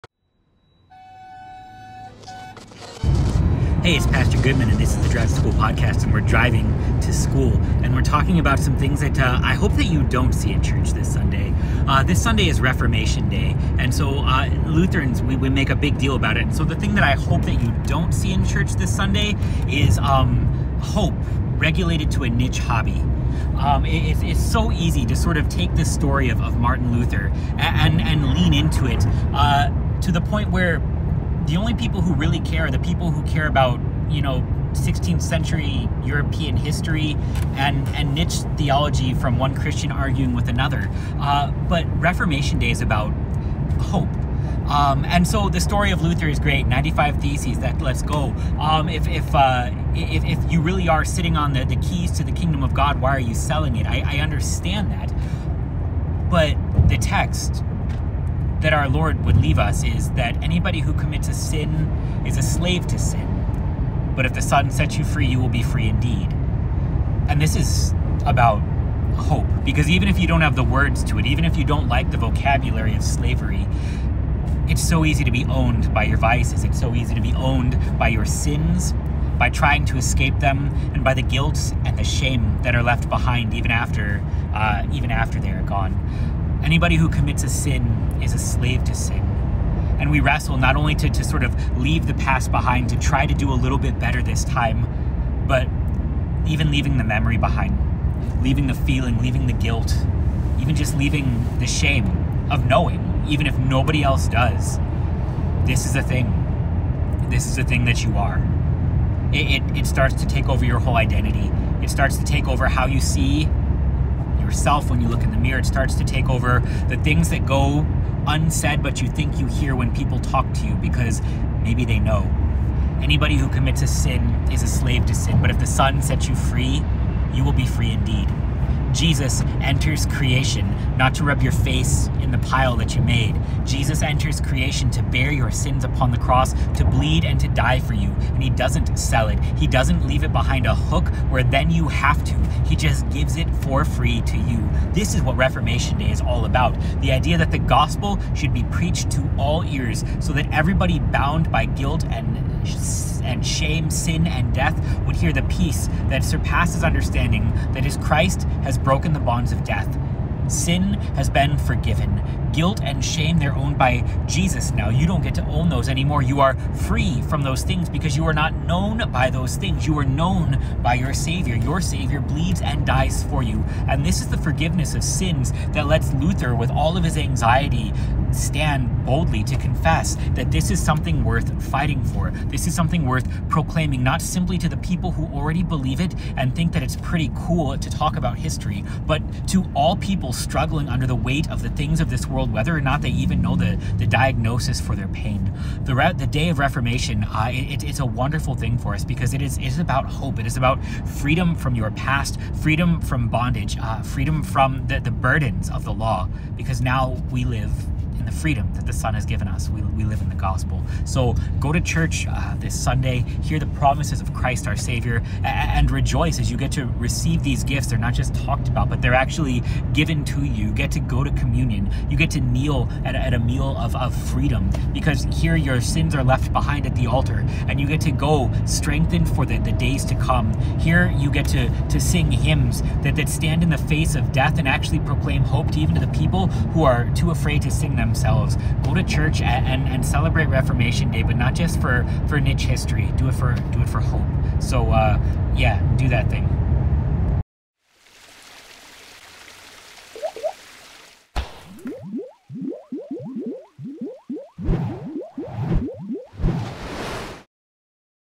church as a hobby?
Pr. Goodman's back to talk about hope vs hobby and why we go to church this Reformation Sunday